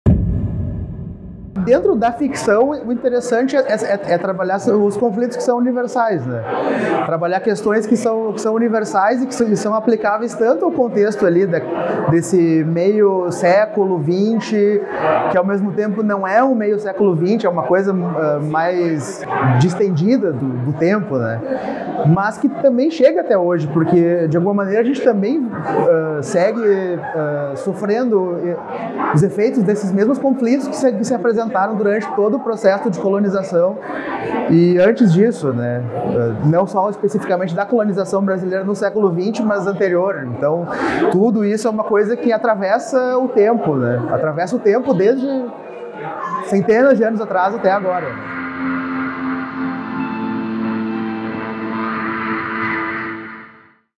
acolmeiafilme. Relatos da Pré Estreia - Matheus Borges - Roteirista 🐝
Relatos da Pré Estreia - Matheus Borges - Roteirista 🐝

A COLMEIA - Uma adolescente luta para fugir da colônia onde vive isolada com um grupo de imigrantes alemães. Traída pelo irmão gêmeo e vivendo nesta atmosfera de pavor, onde o silêncio e a mentira escondem segredos, Mayla tem um desfecho sombrio. Sua carne é doce num tempo amargo e obscuro.